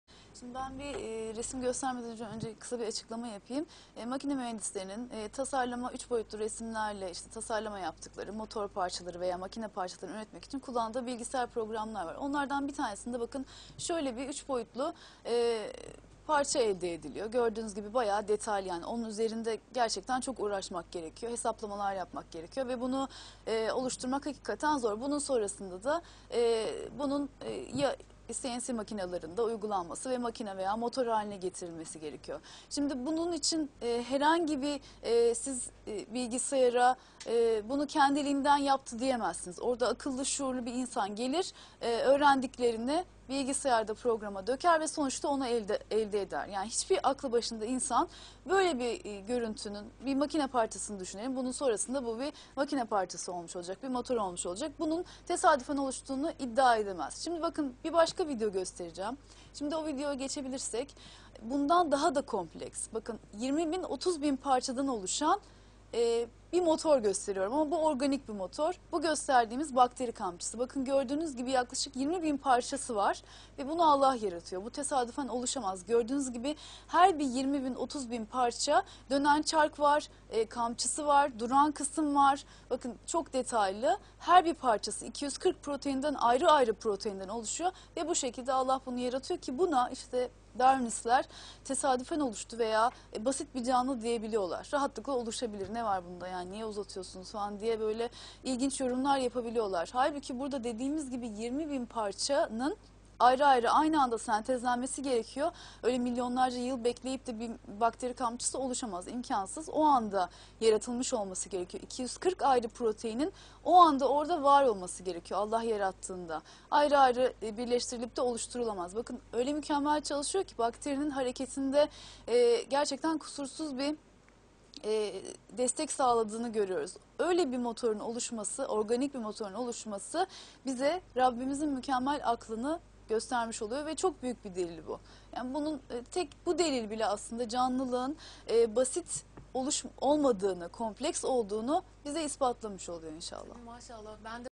20 BİN PARÇASI OLAN BİR ORGANİK MOTOR: BAKTERİ KAMÇISI
GÜLŞAH GÜÇYETMEZ: YAKLAŞIK 20 BİN PARÇASI OLAN BİR ORGANİK MOTOR BAKTERİ KAMÇISI

Gülşah Güçyetmez A9 Tv .